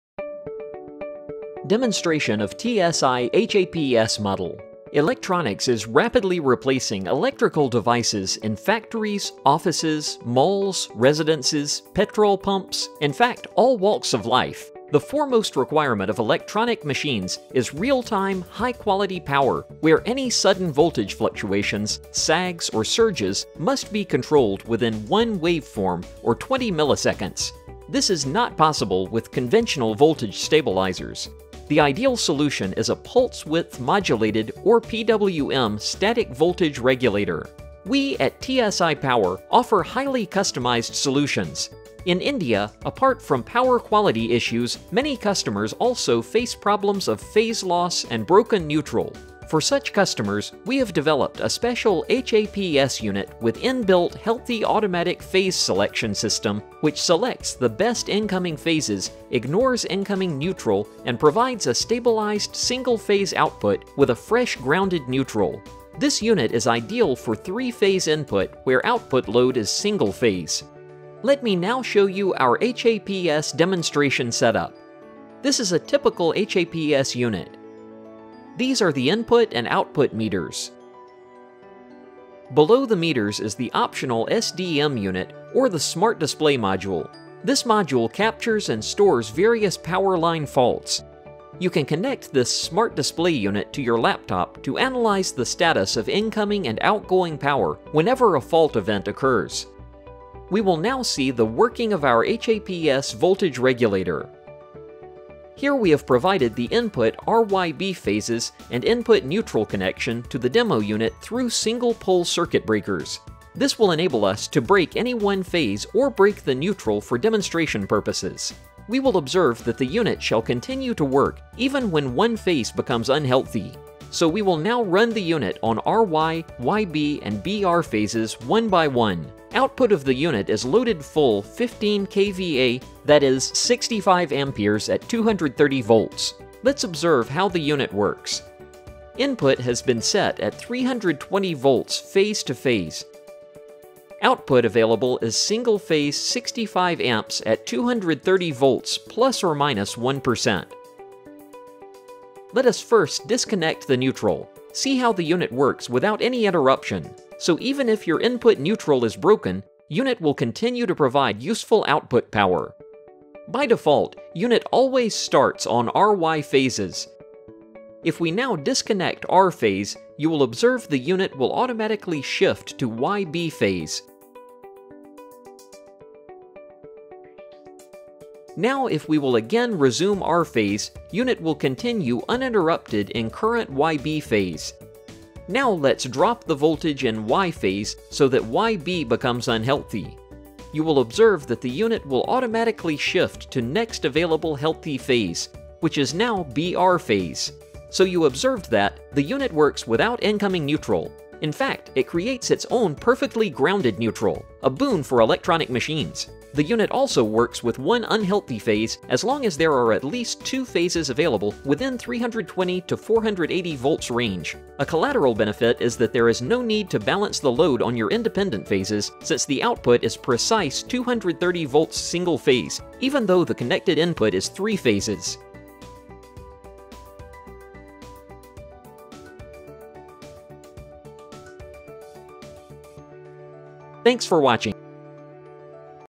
Demonstration of HAPS Model Based on TSi’s Revolutionary PWM Static Voltage Regulation Technology
In today’s time, electronics is rapidly replacing electrical devices in factories, offices, malls, residences, food outlets & petrol pump etc.

The foremost demand of electronics is high quality of power complying to requirement of ITIC Curve, where any sudden voltage fluctuations, surges, sags are controlled within 1 waveform, that is, 20 milliseconds or one fiftieth part of a second.

The ideal solution is PWM static voltage regulator which has the capability to instantaneously correct all kinds of voltage defects, including the very harmful sags.

TSi developed a special unit for areas where the customer faces the following issues:

1. Weak/broken neutral
2. One of the incoming phases being either low or absent.

Watch the entire video to understand the working of this HAPS Model Based On TSi’s Revolutionary PWM Static Voltage Regulation Technology.

To know about the other solutions provided by TSi, please visit TSi Power-India’s